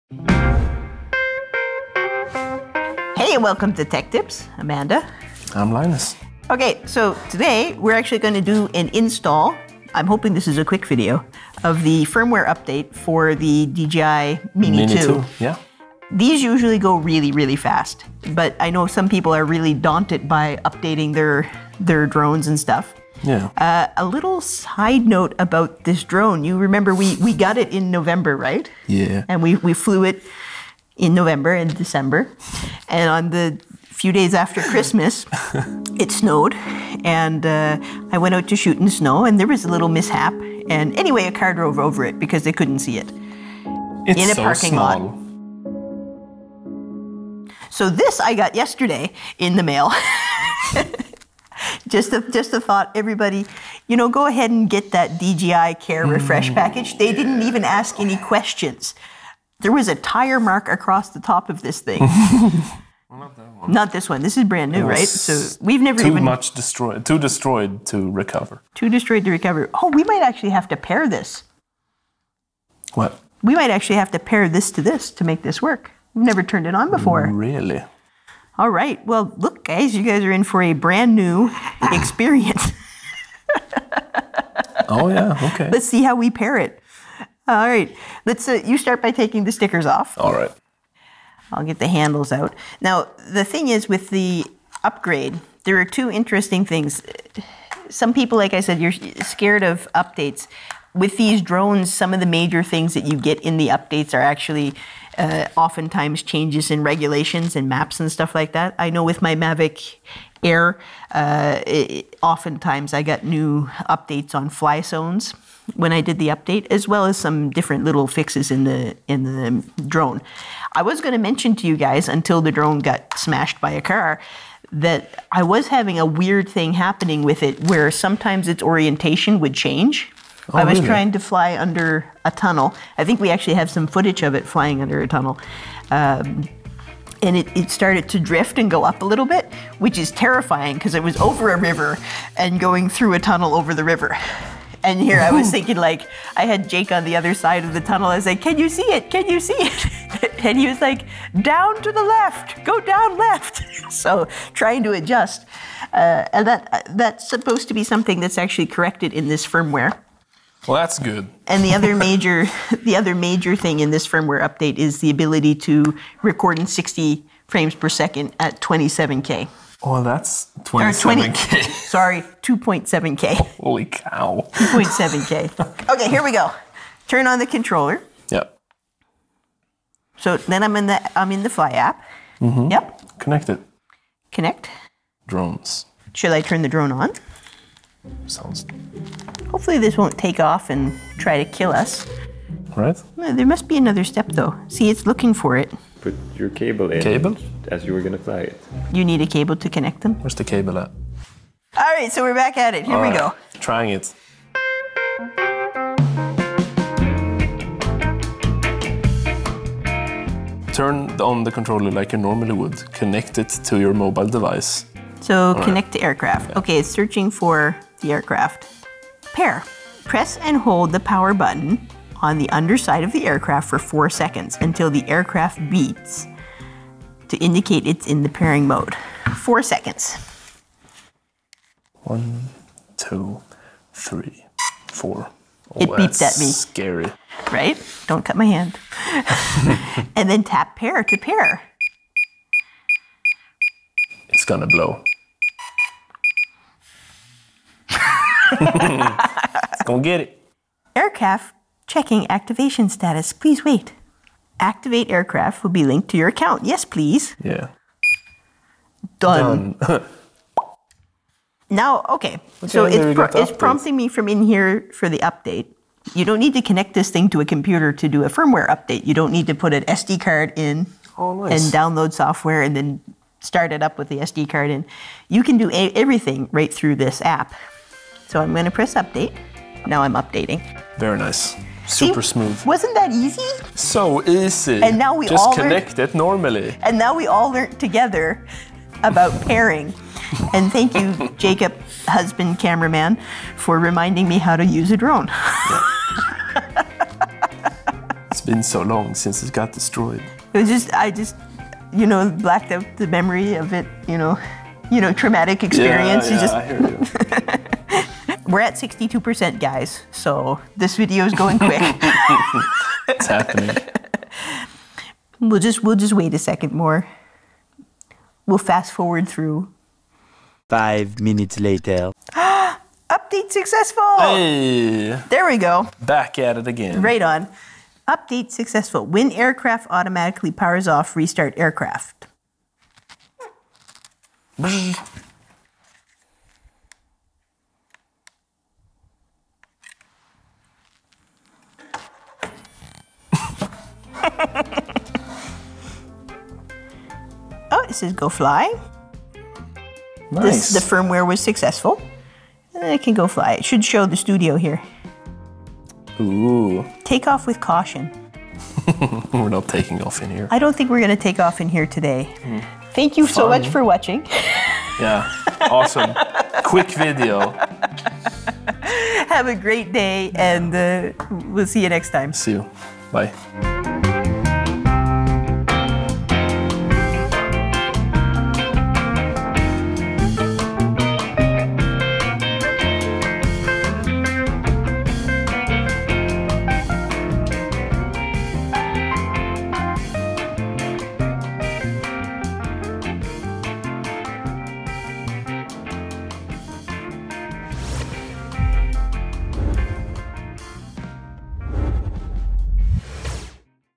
Firmware update and pairing your DJI Mini 2
In today’s tech tips we go over how to do the firmware update from the Dji mini 2 and go over how to pair the controller if for some reason the drone has become unlinked to your controller.